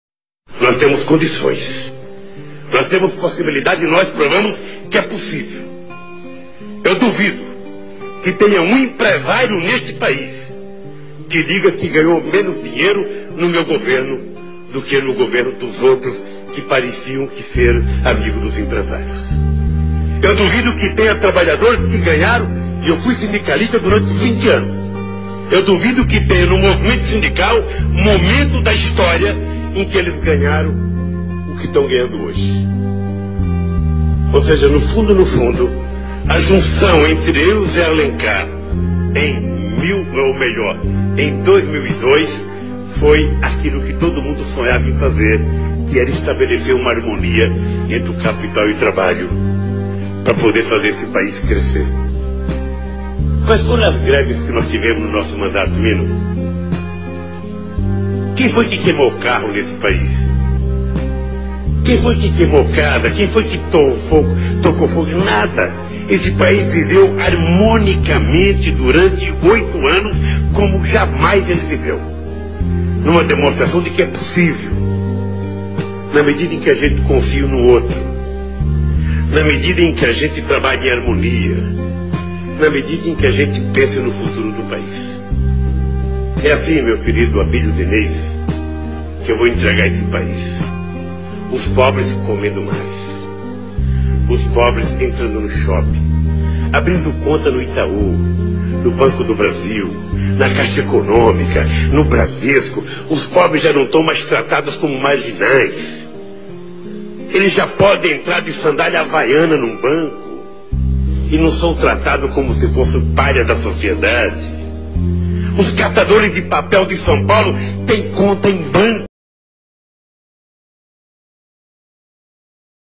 Os ricos ganharam muito dinheiro no governo do PT, o problema foi ascensão do pobre
Vídeo de Aaron Lino